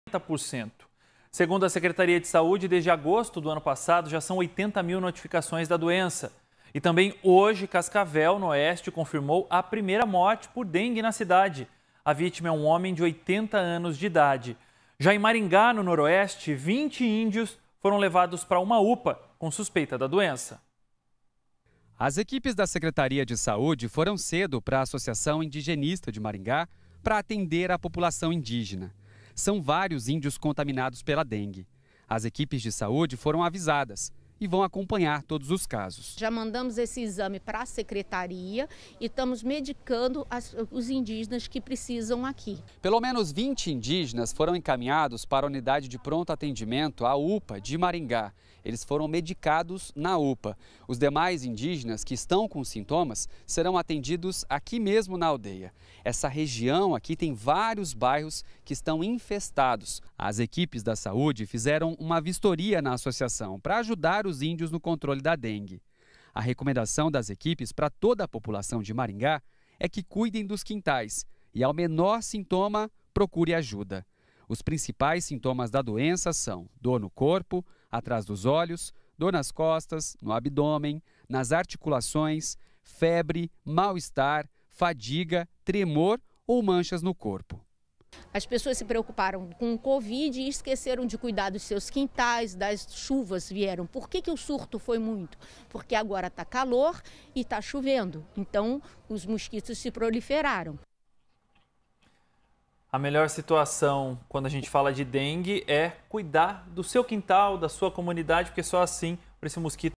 Infestação de mosquitos Assindi provoca surto de dengue entre indígenas
E há uma infestação de mosquitos Assindi e provocou um surto de dengue entre indígenas. Confira todos os detalhes na reportagem!